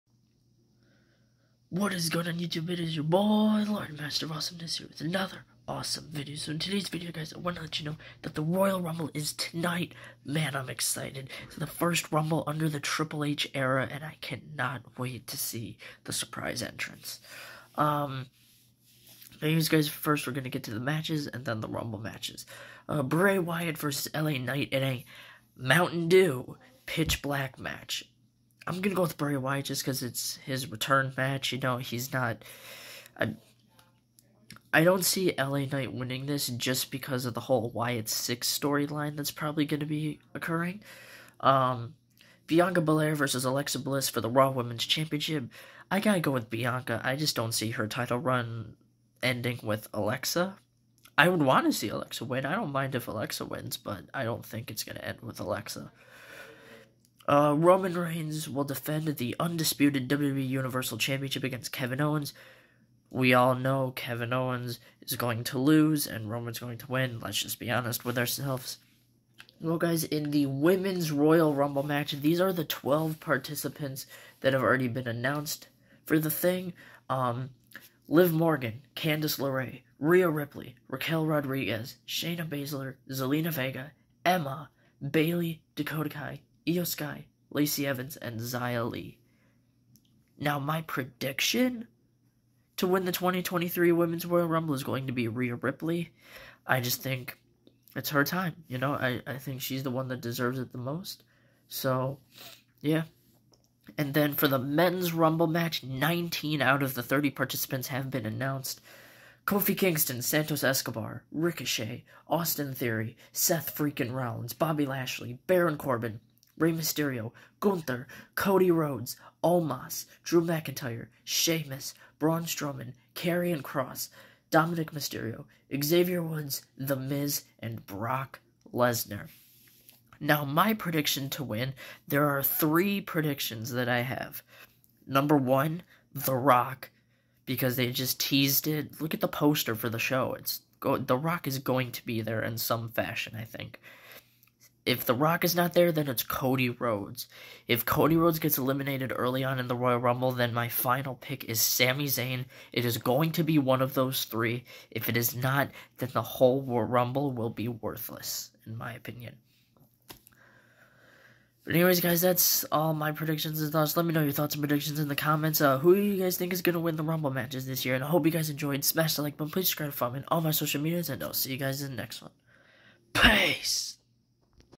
Royal Rumble 2023 PPV Predictions: THEROCK!!!!!!!
Bray Wyatt vs LA Knight in a Mountain Dew Pitch Black Match

Bianca Belair vs Alexa Bliss for the RAW Women’s Championship

Roman Reigns vs Kevin Owens for the Undisputed WWE Universal Championship

Liv Morgan vs Candice LeRae vs Rhea Ripley vs Raquel Rodriguez vs Shayna Baszler vs Zelina Vega vs Emma vs Bayley vs Dakota Kai vs Iyo Sky vs Lacey Evans vs Xia Li vs ? vs ? vs ? vs ? vs ? vs ? vs ? vs ? vs ? vs ? vs ? vs ? vs ? vs ? vs ? vs ? vs ? vs ? in a Women's Royal Rumble Match

Kofi Kingston vs Santos Escobar vs Ricochet vs Austin Theory vs Seth Rollins vs Bobby Lashley vs Baron Corbin vs Rey Mysterio vs Gunther vs Cody Rhodes vs Omos vs Drew McIntyre vs Sheamus vs Braun Strowman vs Karrion Kross vs Dominik Mysterio vs Xavier Woods vs The Miz vs Brock Lesnar vs ? vs ? vs ? vs ? vs ? vs ? vs ? vs ? vs ? vs ? vs ? in a Men's Royal Rumble Match